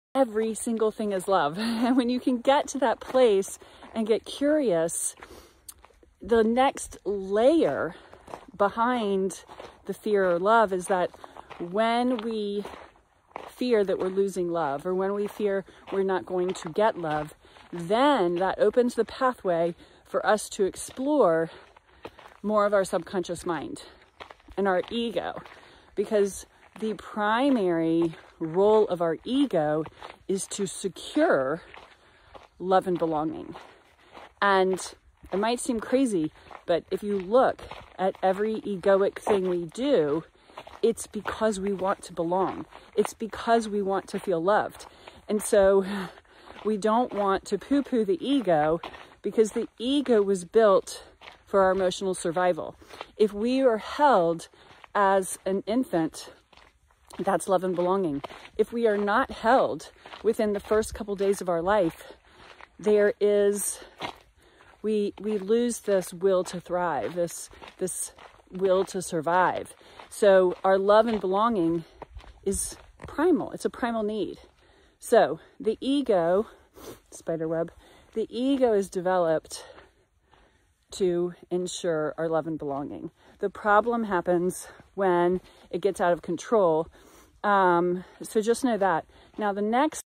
Awaken Your Power: REDIRECT the Ego to Align, Elevate, and Accelerate Your Life!
The ego is essential to our emotional survival…

Check out my Self Sabotage Course: A User’s Guide to the Subconscious Mind